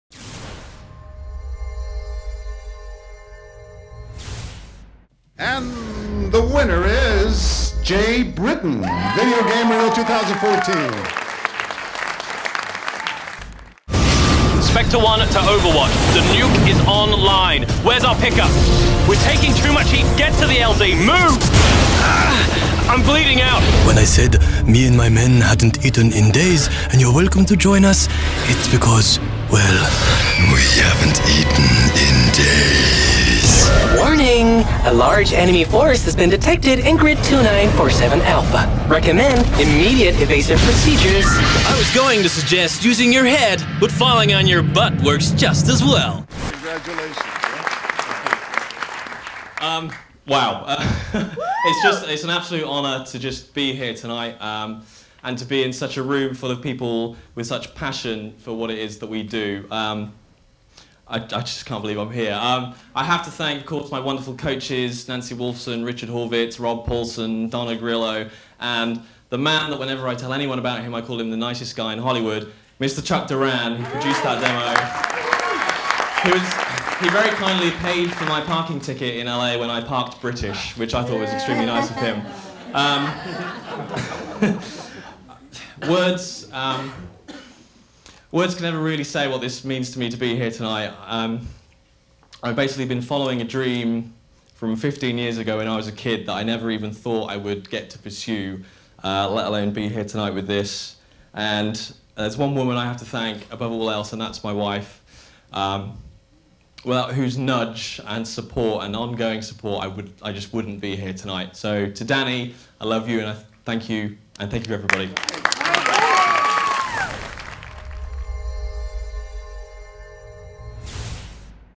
Jay Britton - Outstanding Animation Demo
Voice Arts™ Awards 2014
Outstanding Animation Demo Reel
Video Game Reel 2014
Voice Artist: Jay Britton
Producer: Chuck Duran